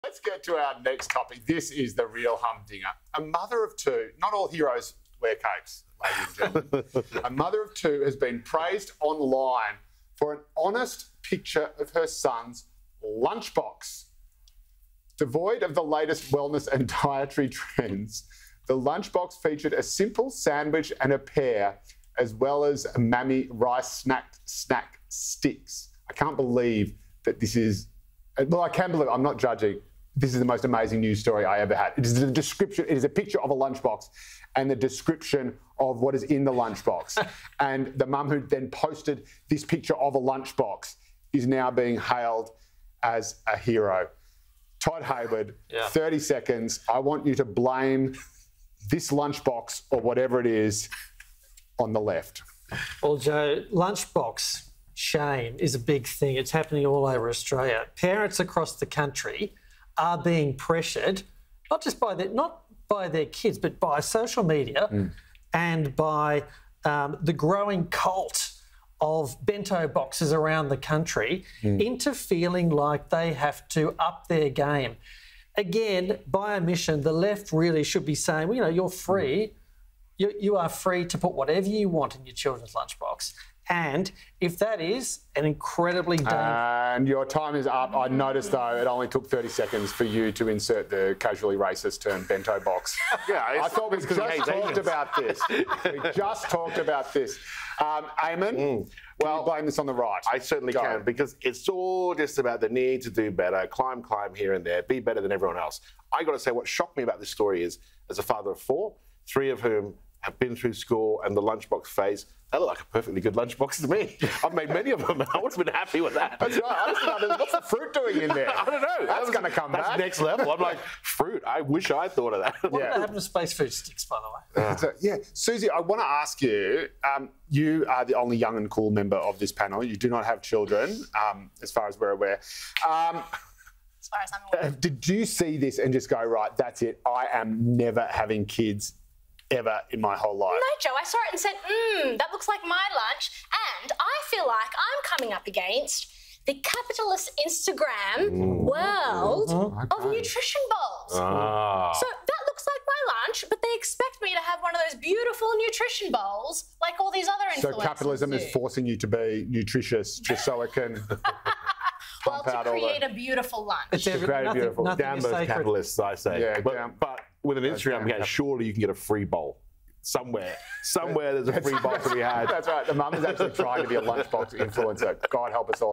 ‘Lunch box shame’ is a big thing in Australia
Telamon Consulting Director Todd Hayward says “lunch box shame” is a big thing and it’s “happening all over Australia”.

“Parents across the country are being pressured not by their kids but by social media … into feeling like they have to up their game,” he told Sky News host Joe Hildebrand.